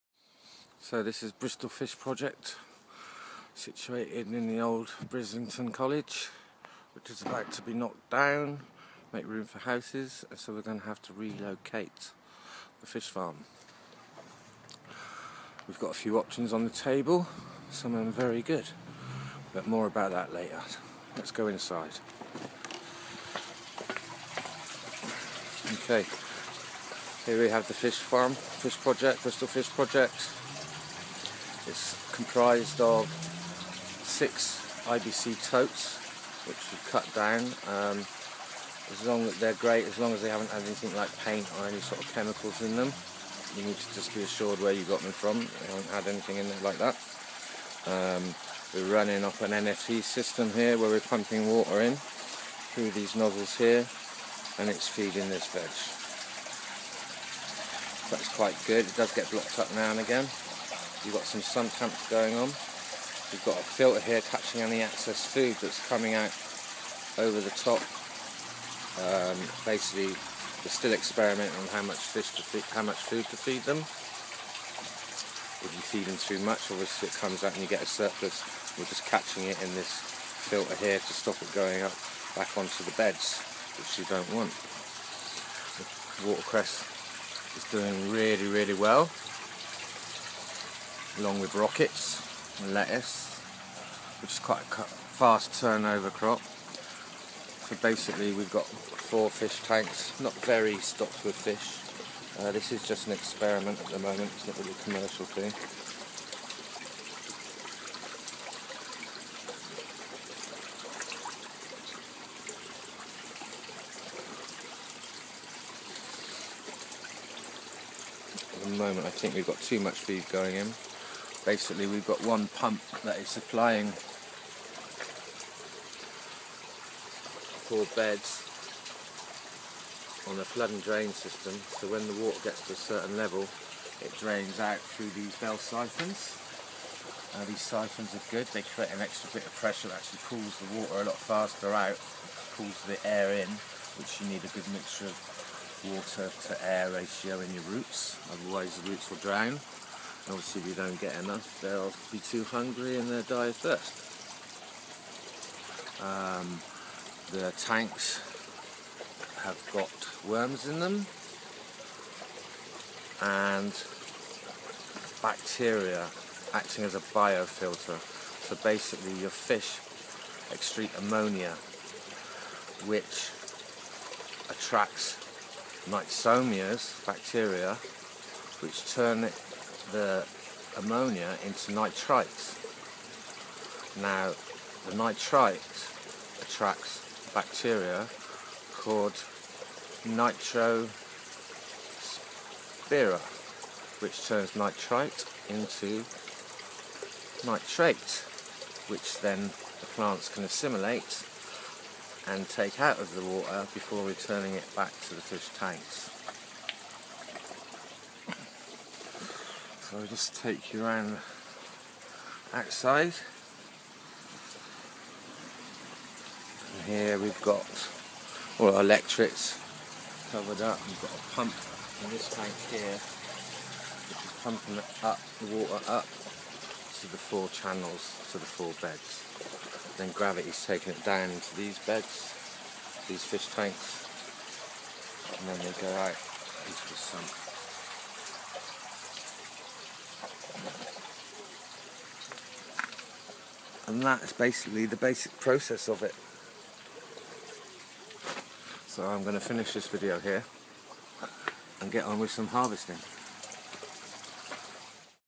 Bristol Fish Project - The Nitrification Process
A brief explanation of the Nitrification process & the flood & drain system used by Bristol Fish Project by Rob Bryant